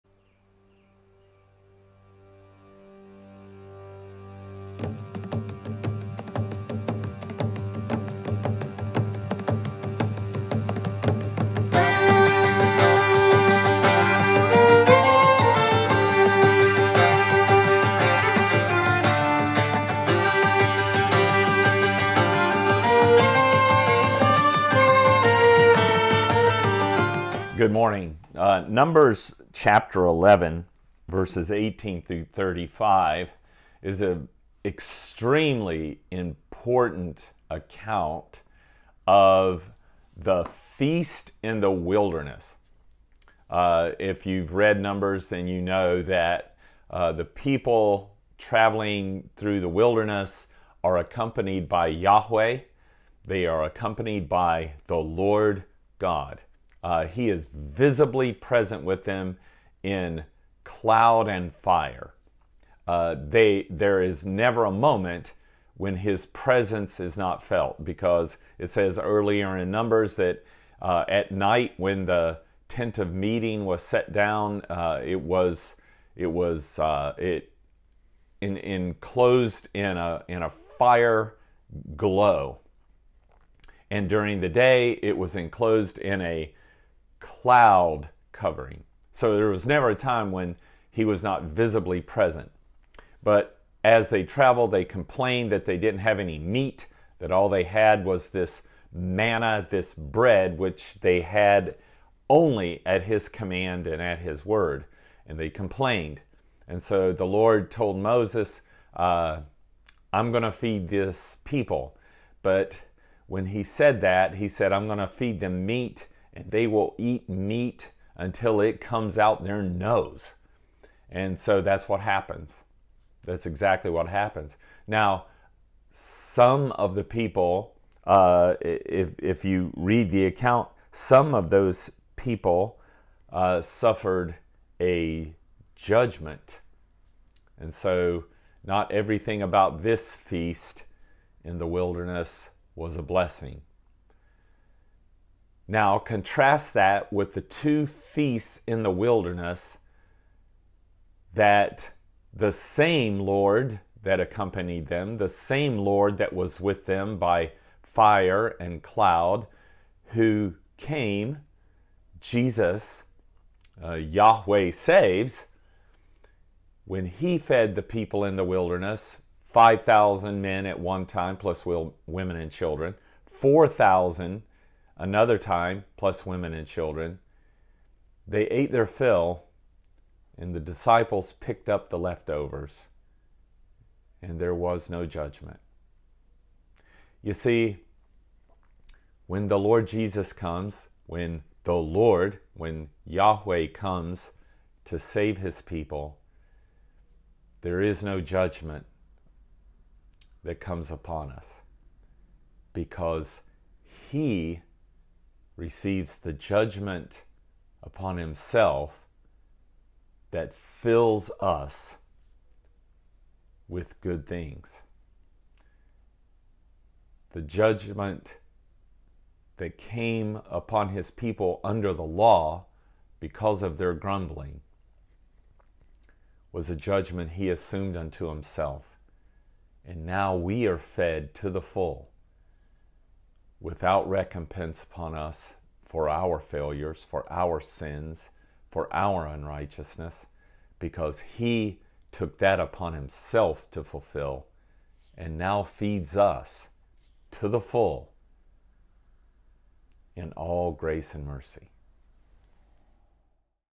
Fed without Judgment
Tuesday: In the Garden before sin, God fed us without Judgement.  Under the Law, even God's provision was under the Judgment brought by our sin,  In Christ, we feast without Judgment again...forever.